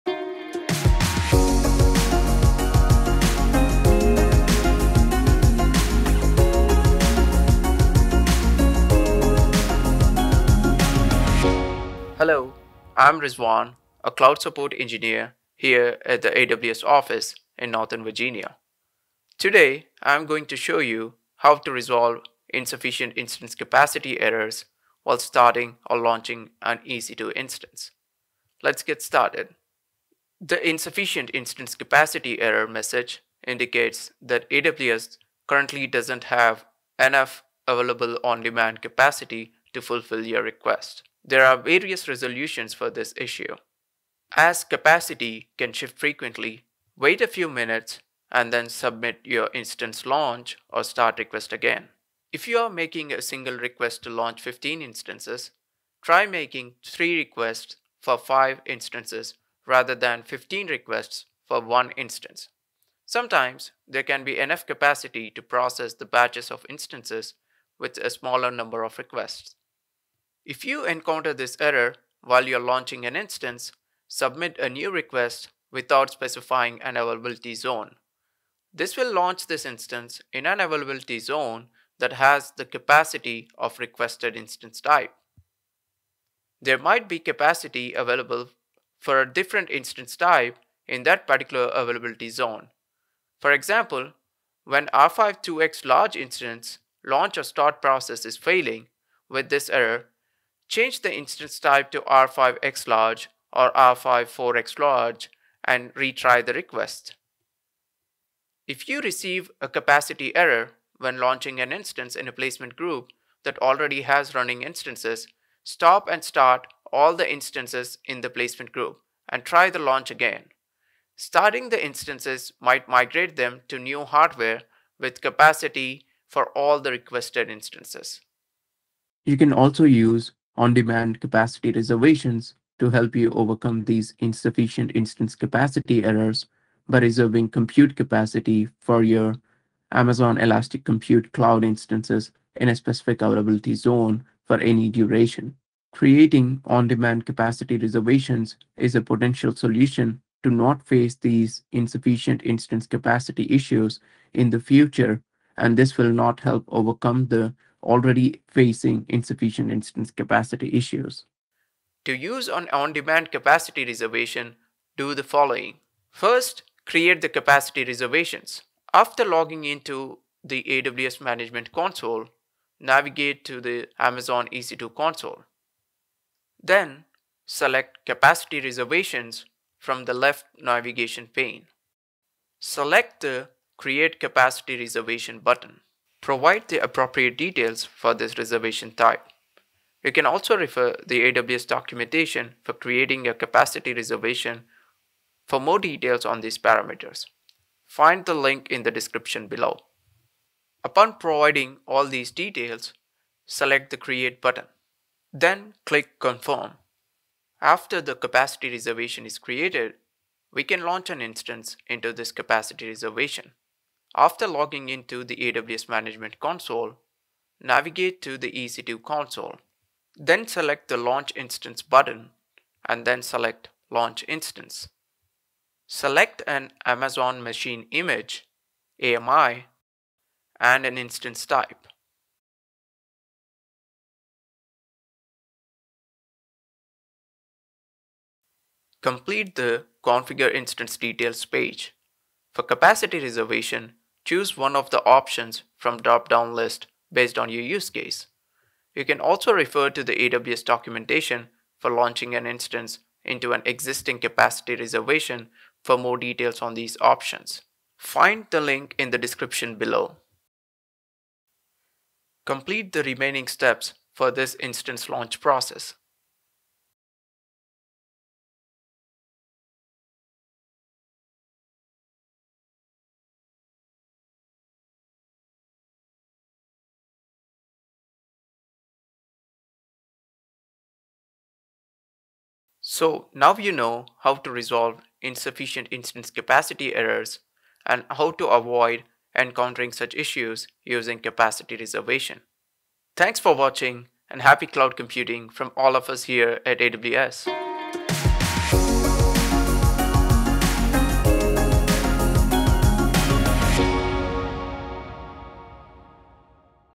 How do I troubleshoot InsufficientInstanceCapacity errors when I start or launch an EC2 instance?
For more details see the Knowledge Center article with this video

0:00 Intro
0:31 Starting notes
2:45 Create the Capacity Reservations
3:27 Launch instance into Capacity Reservation
4:53 Ending